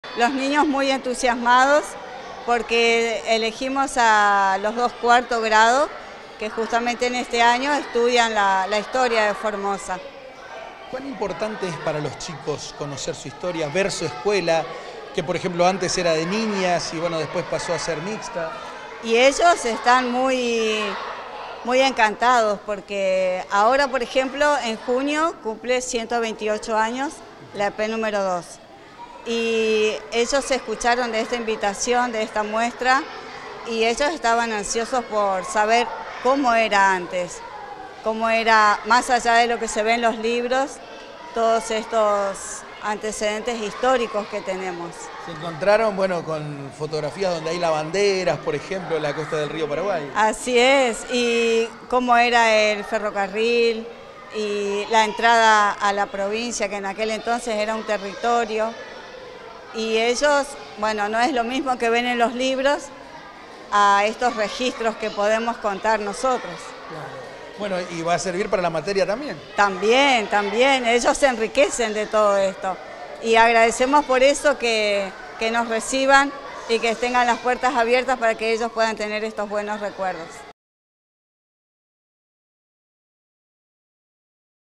ESTUDIANTES DE 4° GRADO PARTICIPARON DE LA MUESTRA FOTOGRÁFICA
COMENZÓ LA PRIMERA ETAPA DE LA MUESTRA FOTOGRÁFICA “IMÁGENES PARA PENSAR EL PASADO”. ESTUDIANTES DEL 4° GRADO DE LA ESCUELA PRIMARIA N° 2, VISITARON LA MUESTRA FOTOGRÁFICA Y ENRIQUECIERON AÚN MÁS SU CONOCIMIENTO SOBRE LA HISTORIA DE LA CIUDAD DE FORMOSA.